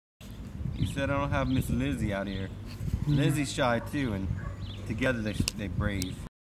Rolo
Rolo a male Hound mix available for adoption.